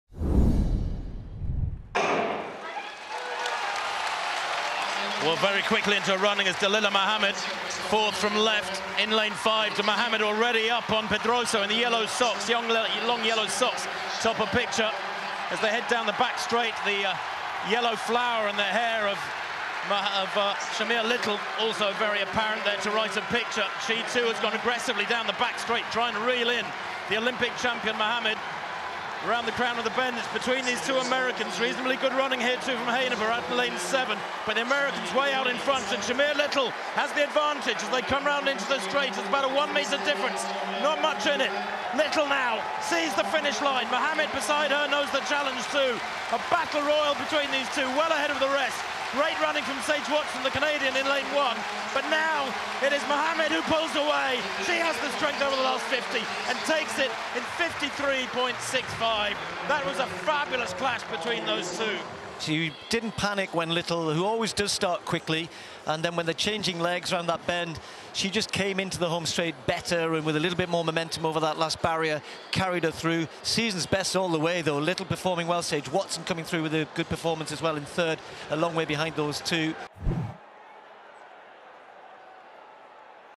Dalilah Muhammad Wins Women's 400m Hurdles - IAAF Diamond League Oslo 2018
Top 3 results:
1. Dalilah Muhammad (USA) - 53.65 SB
2. Shamier Little (USA) - 53.94 SB
3. Sage Watson (Canada) - 54.55 SB